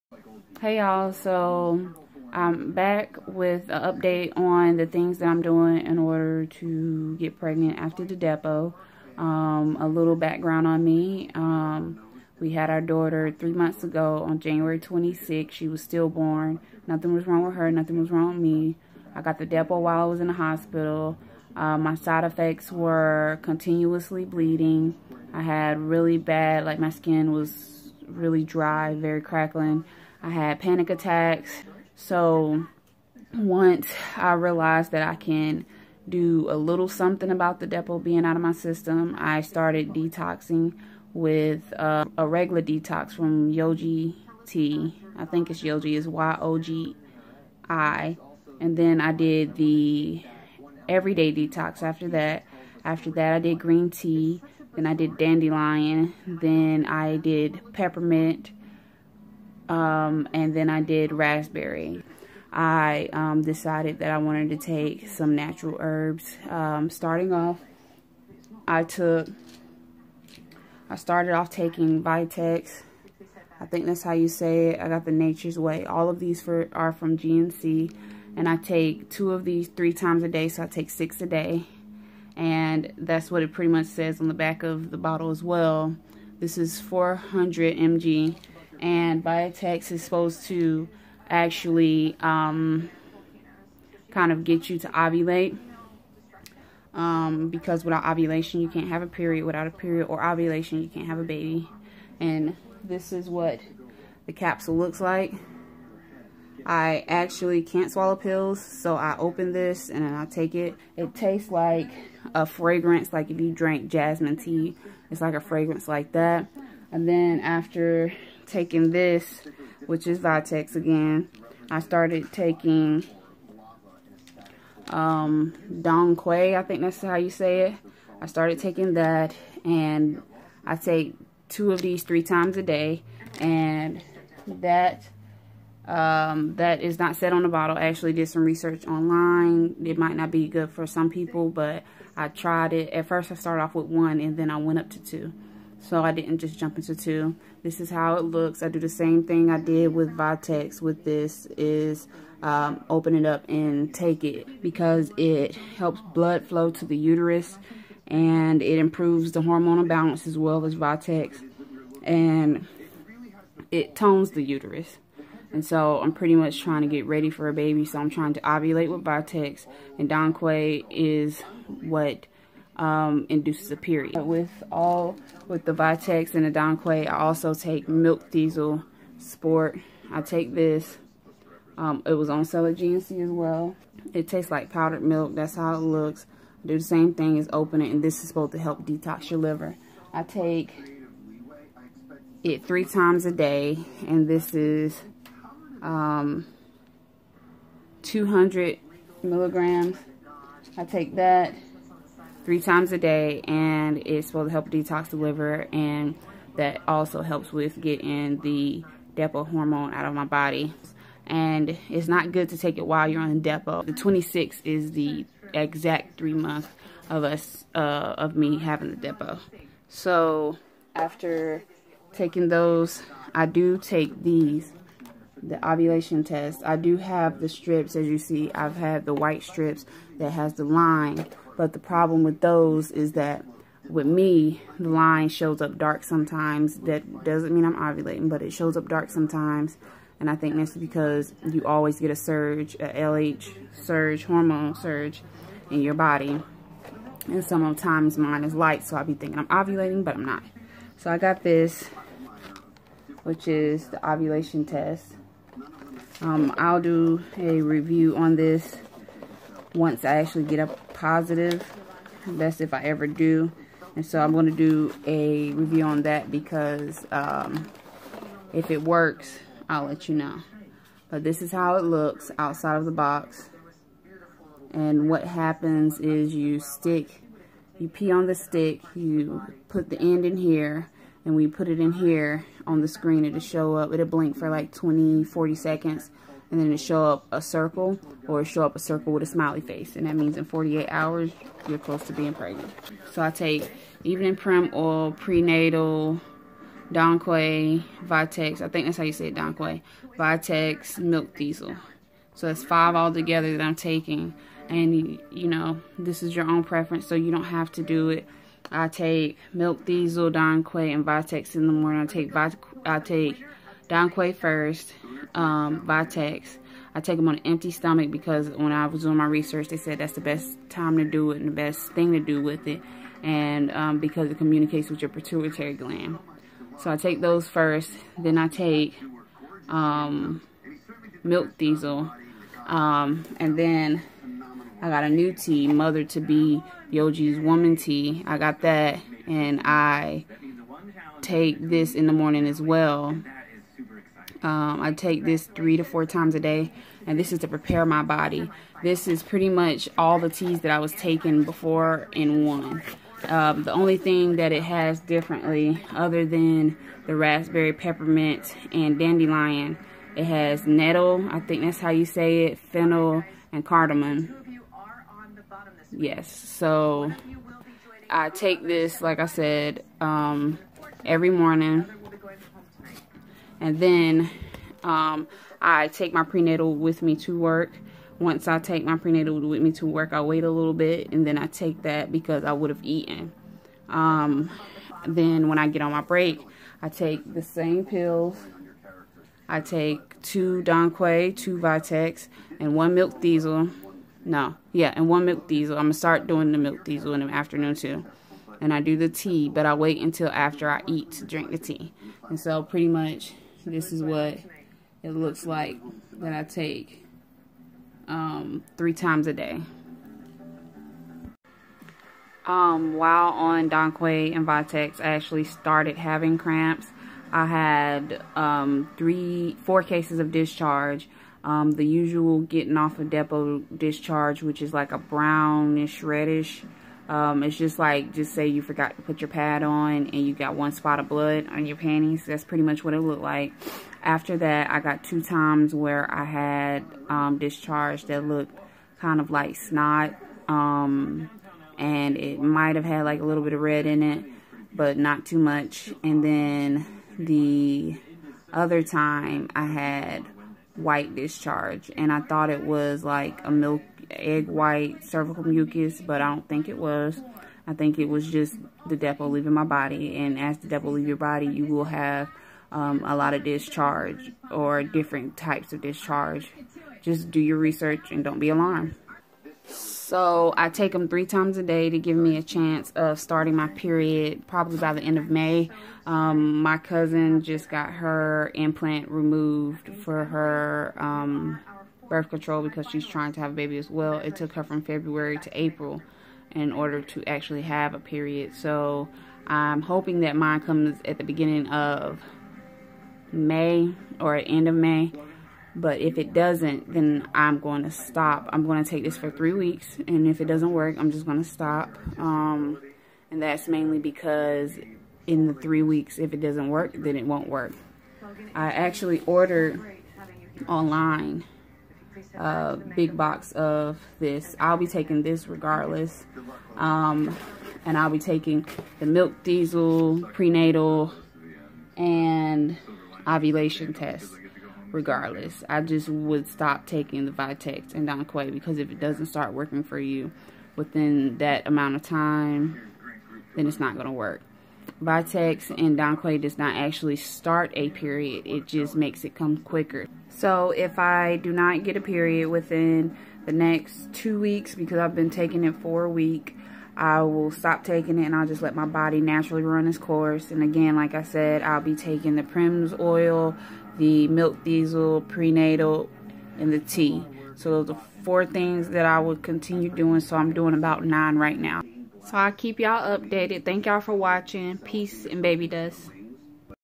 Getting pregnant after depo - dong quai, vitex, milkthistle, and mother to be tea
I got the depo on January 27th after our daughter was still born. I  had the side effect of continues bleeding, dry skin, and panic attacks. After taking primrose oil as well as detox tea the bleeding stopped. Then i took a course of milkthistle ,everyday detox tea, green tea, dandelion, peppermint, and raspberry. I started on the 22nd and will take for 2 more weeks to see if dong quai and vitex will induce a period